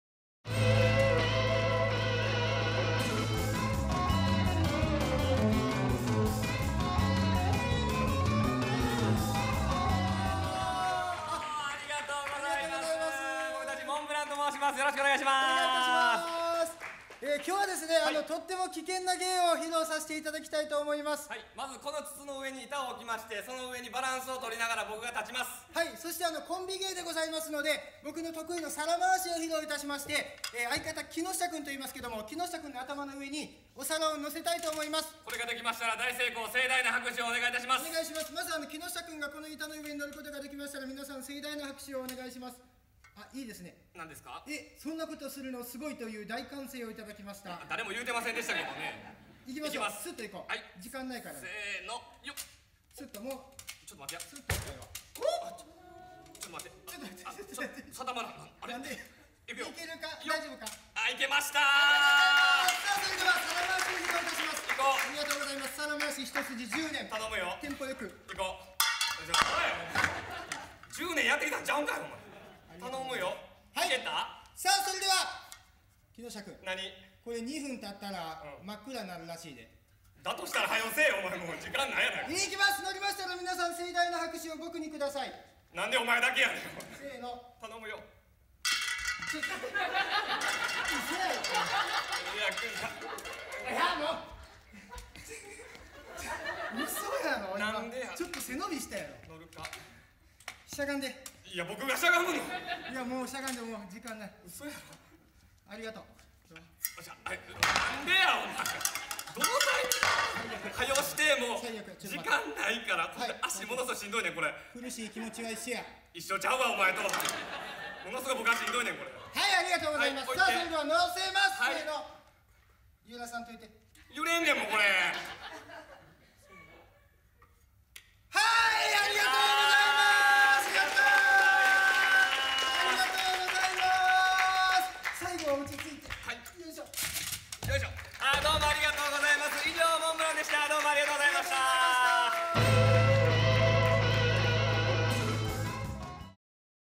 【よしもと漫才劇場お披露目公演】モンブラン
【コンビ】
モンブラン　2011年結成

【個人名】
いけっち　木下

【意気込み】
元気一杯頑張ります！